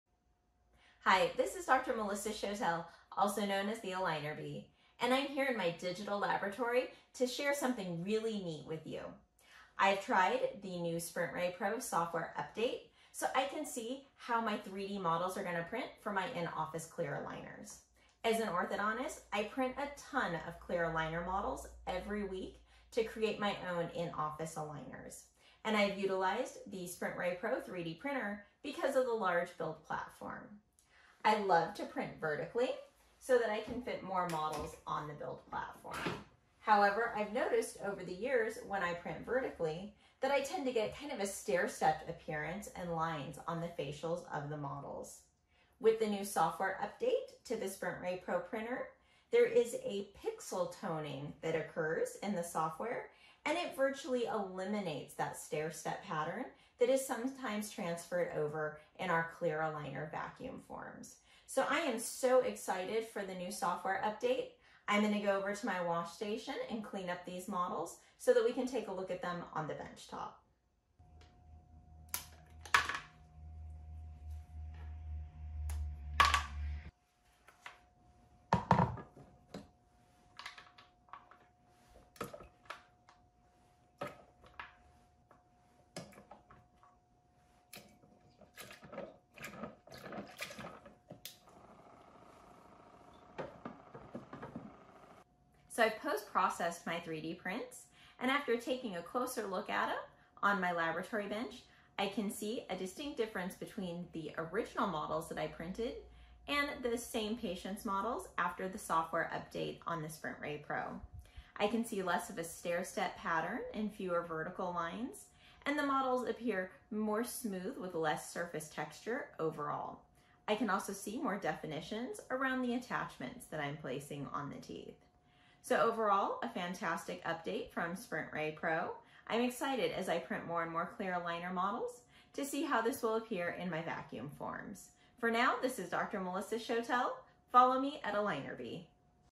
SprintRay Pro Software Update - Review
The new SprintRay Pro 3D Printer Software update has some fantastic new improvements for 3D printing clear aligner models.  Check out videos and photos of the new models with the Pixel Toning technology!